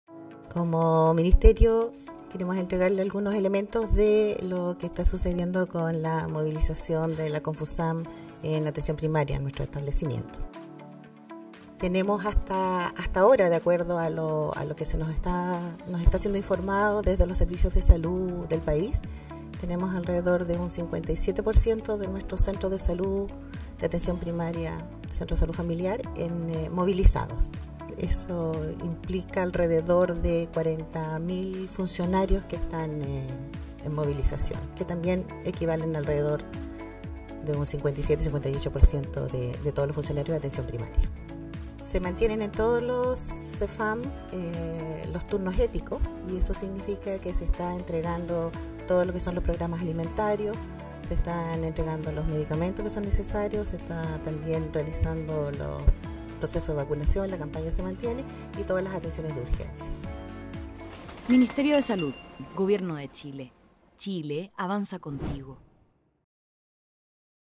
Jefa de la División de Atención Primaria del Minsal por paro de la Confusam
Jefa de la División de Atención Primaria del Minsal, Carmen Aravena, se refiere a la movilización que realizó la Confusam durante la jornada del jueves 18 de abril del año 2024.